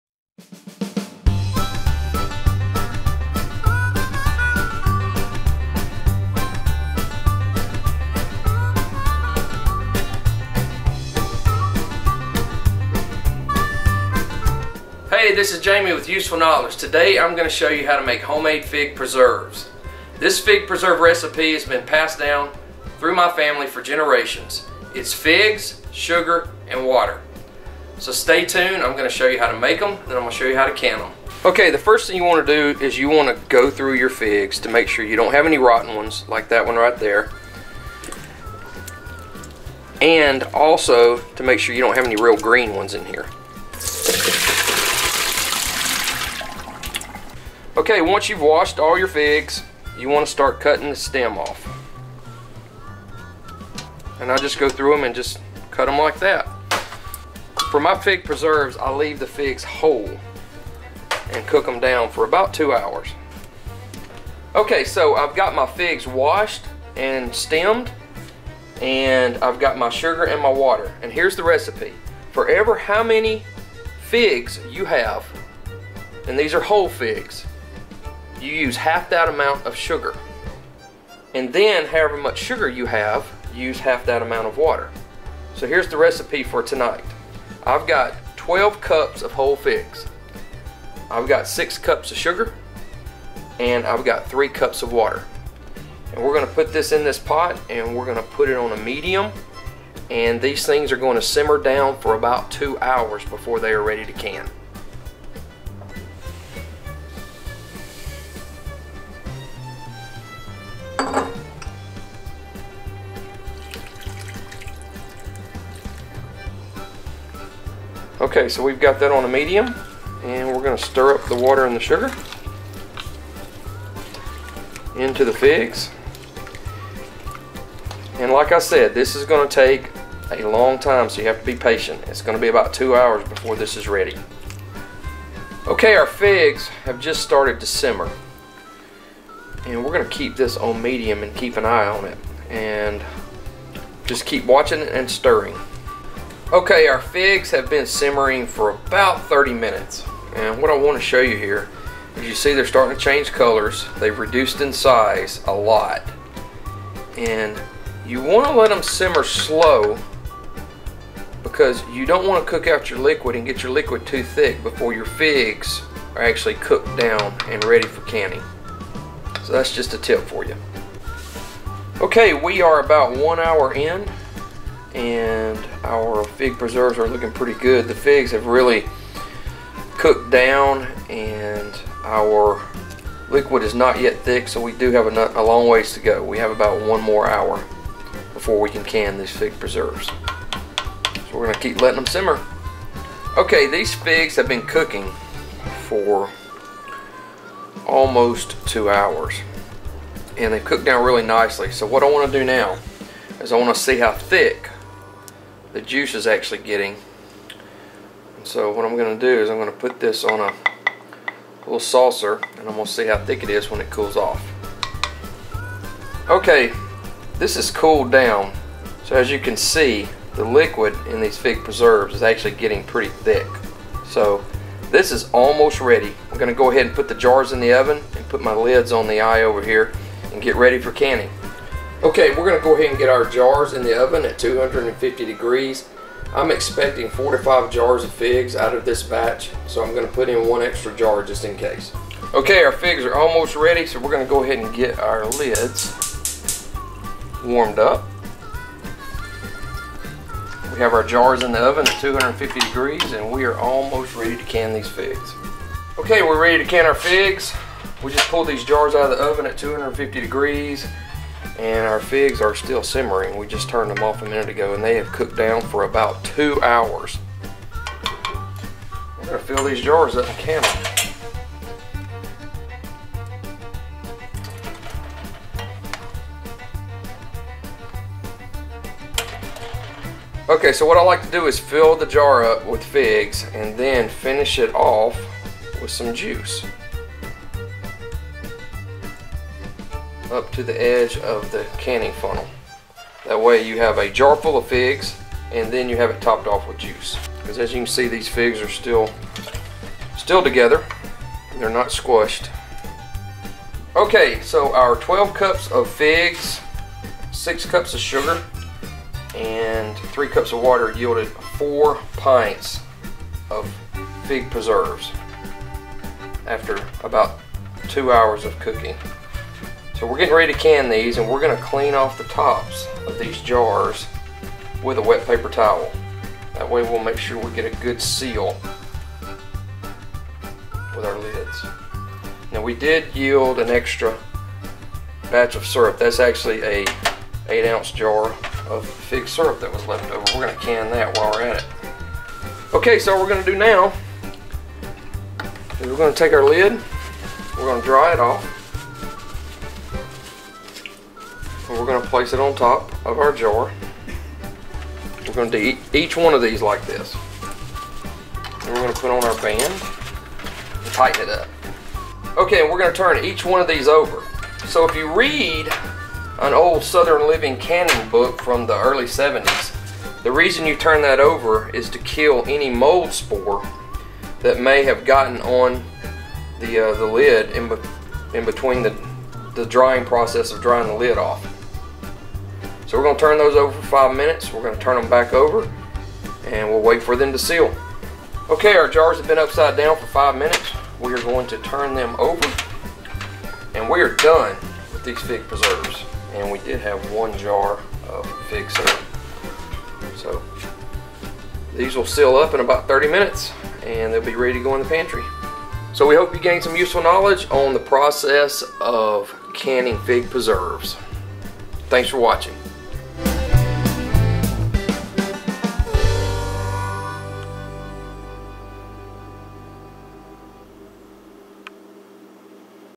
Fig Preserves - The Old Fashioned Way! | Useful Knowledge
Fig preserves are simple and easy to make.  Old fashioned fig preserves are made from figs, sugar, water.  Our fig preserves recipe was passed down through our family for generations.  These preserves are made by slow simmering the figs, sugar, and water. We show you how to easily make and can them.

Real old fashioned fig preserves require about 2 hours of simmer time.  Then canning fig preserves is quick and simple.    We show you how easy it is to make and can these preserves.  No water bath. I had forgot about making fig preserves in years until a couple of years ago.  After making a batch, I planted 4 fig trees of my own.

If you have only made the fake strawberry preserves using figs, you really should try this recipe.  This is truly old fashioned and the fig preserves have a unique flavor that is awesome on biscuits.

The taste of these old fashioned fig preserves will make you want to make these every summer.  My grandparents made these preserves until the kitchen was full of canned fig preserves. The simple and easy fig preserve recipe is include in the video.

This is not fig jam.  With these fig preserves the figs are left whole and allowed to cooked down to make preserves.  Bring on the southern biscuits and let's eat!

Man with the Knowledge: Jamie Hardy
Editor: John Andrews (UberonN)
Creator: Mark Davies